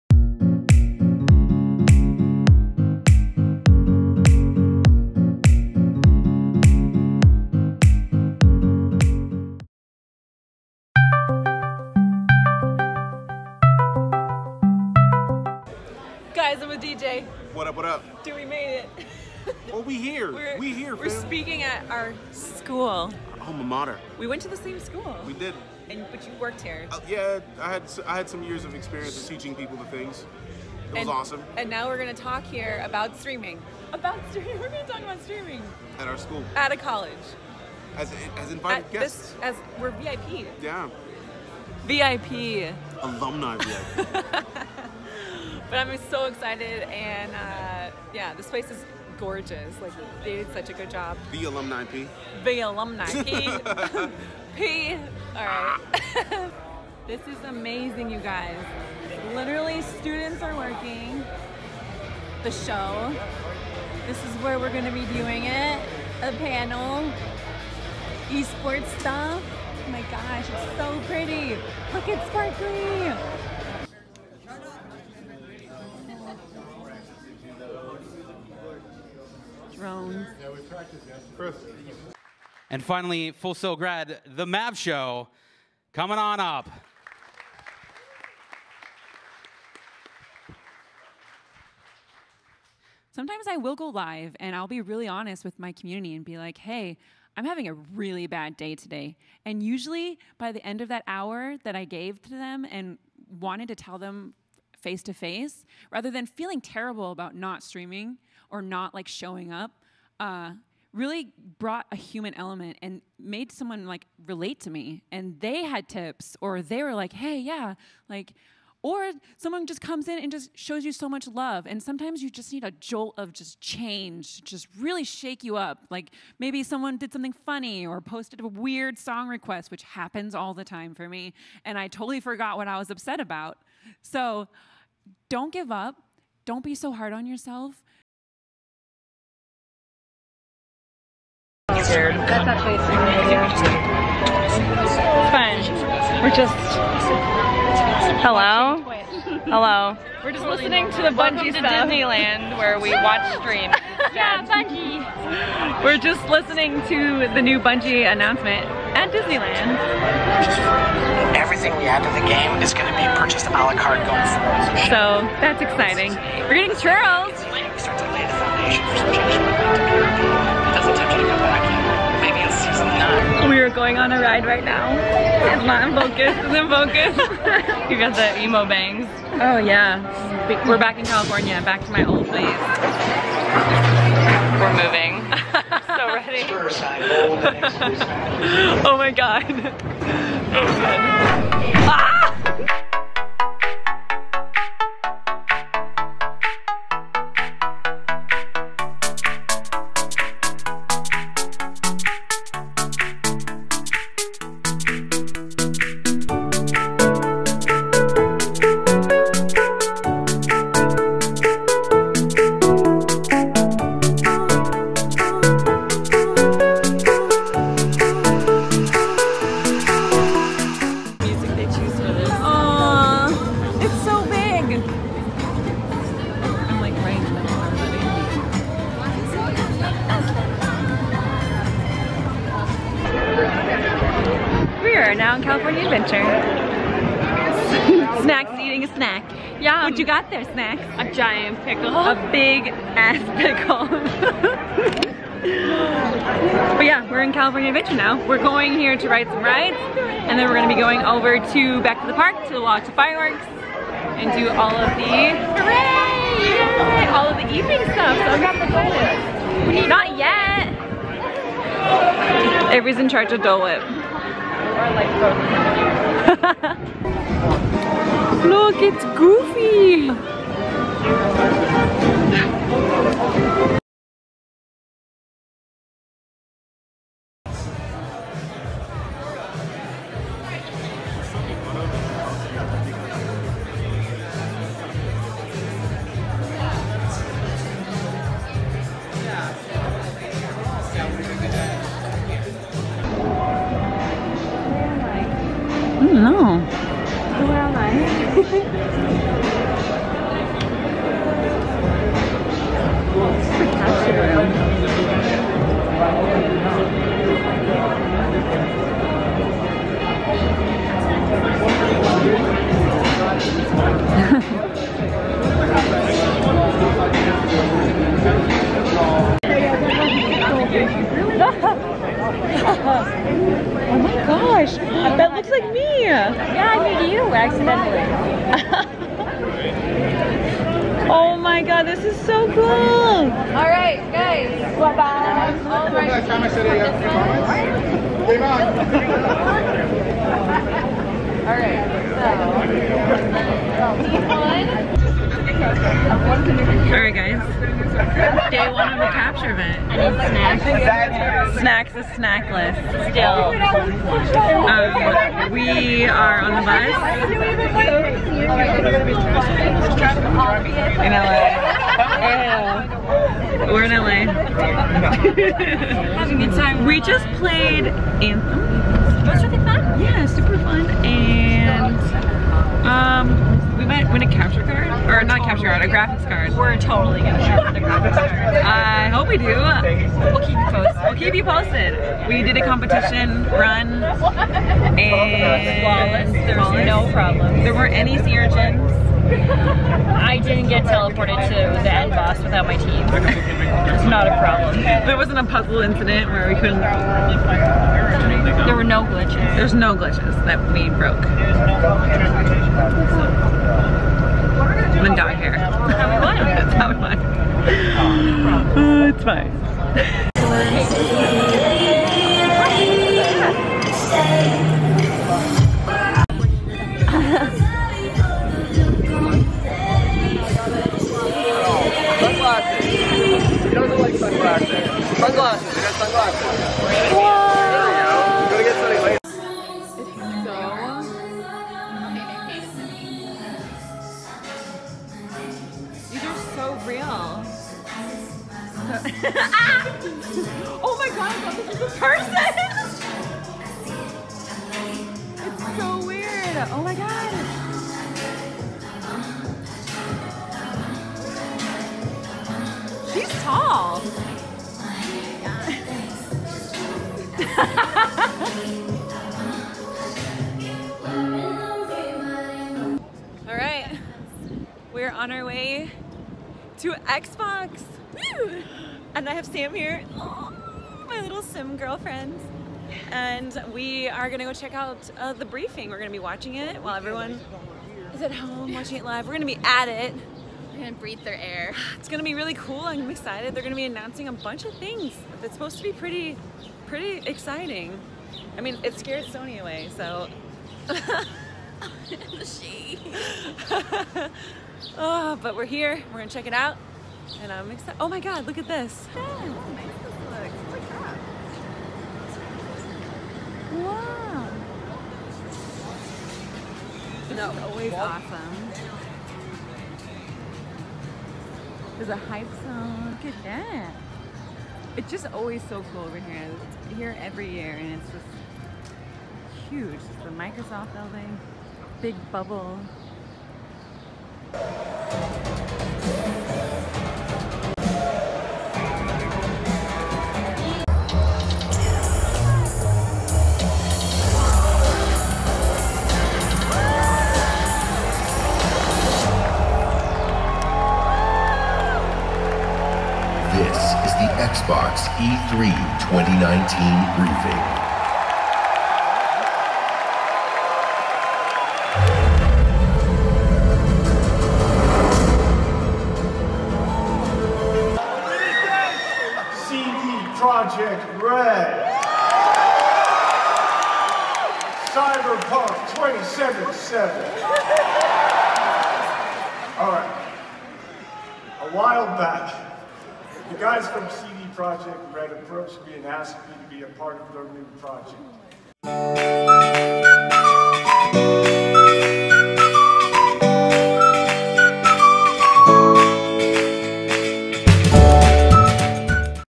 Keanu, EA Play, DisneyLand! | E3 2019
Here is our June 2019 Vlog!!! We saw Keanu at the xbox conference, went to disneyland, was invited to ea play and the summit and just so much friendship time. Happy National Video Game Day!

Green Screen
Stream Deck
What Camera I use